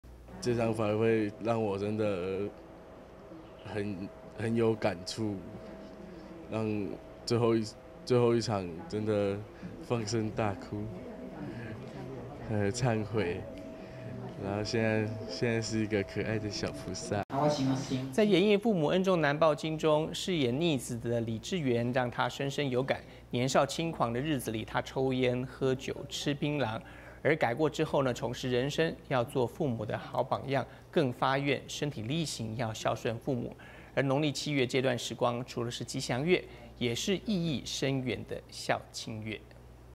高雄祈福會 分享感動
高雄的慈濟志工來到花蓮慈濟靜思精舍分享這次舉辦祈福會演譯父母恩重難報經的體會。志工說從排練到演出，可是感動萬分，更深刻體會 「行善、行孝要及時」。

在演譯父母恩重難報經中飾演「逆子」的李智源，讓他深深有感。年少輕狂的日子裡，他抽菸、喝酒、吃檳榔；改過之後重拾人生，要做父母的好榜樣，更發願身體力行，孝順父母。農曆七月，這段時光除了是吉祥月，也是意義深遠的孝親月。

靜思鼓隊隊員 李智源：「這場法會 讓我真的很有感觸，讓最後一場‧‧‧真的，放聲大哭，很懺悔。現在是一個可愛的小菩薩。」

楊艾庭 蘇國志 翁國嘉 花蓮報導